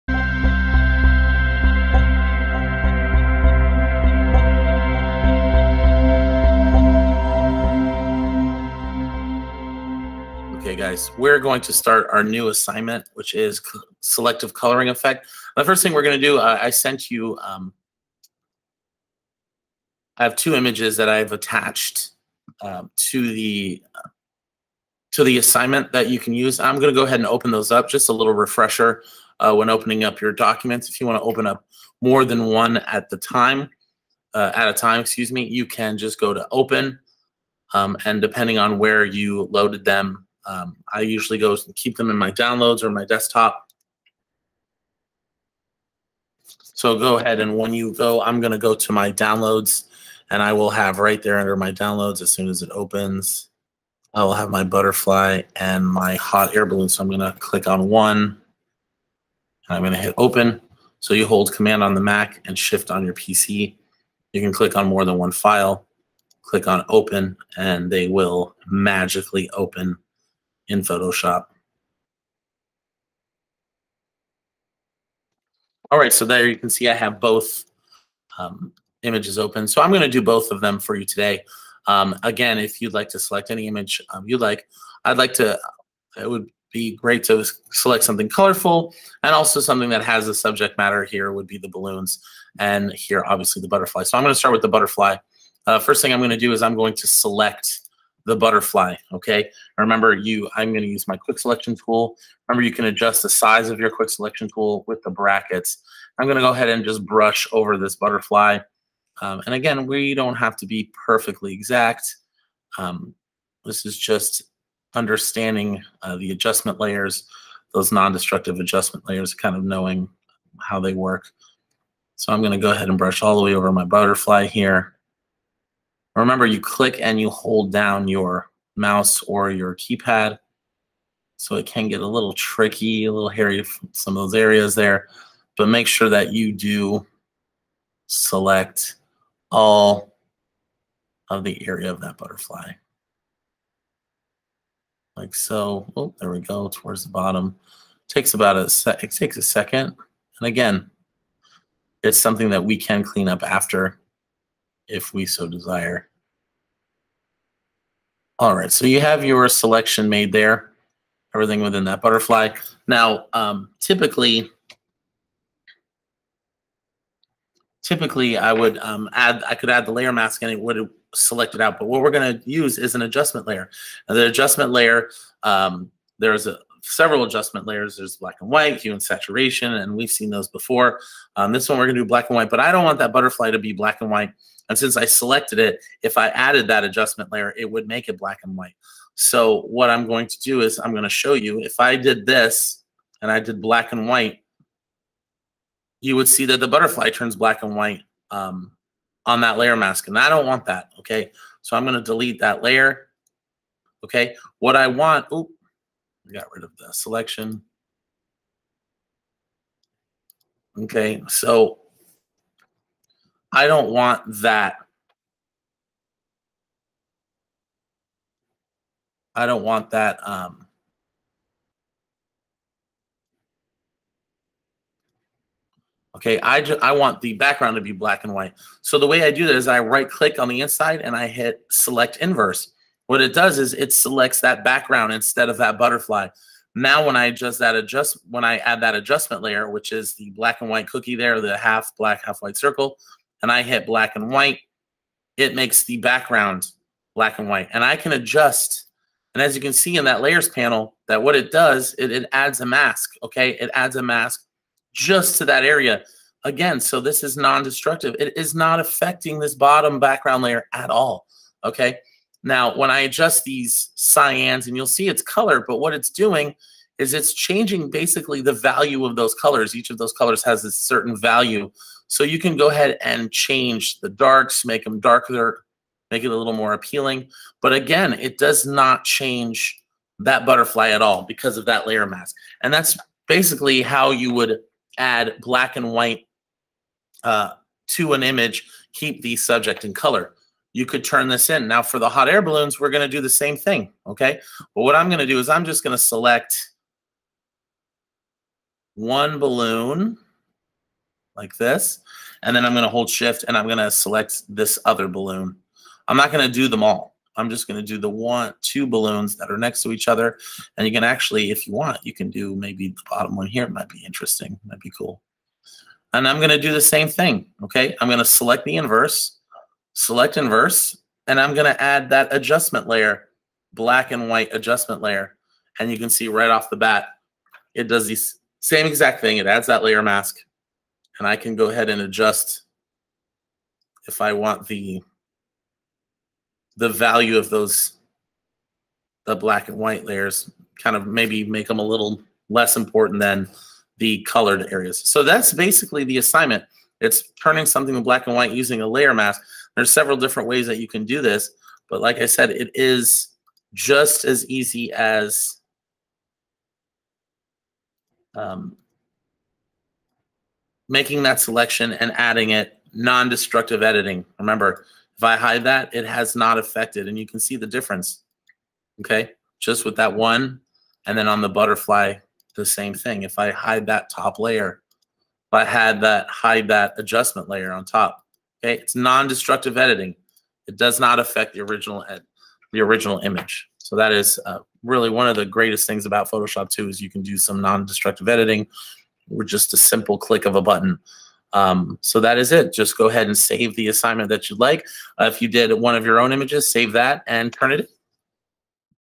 DIGITAL MEDIA - SELECTIVE COLOR EFFECT
In this Photoshop tutorial, we’ll learn an easy way to create a popular selective coloring effect, converting a photo to black and white while keeping the main subject in full color. If that sounds complicated or time consuming, it’s not. In fact, as we’ll see, the entire effect can be created in just a few short steps using nothing more than a selection tool, an adjustment layer and a layer mask!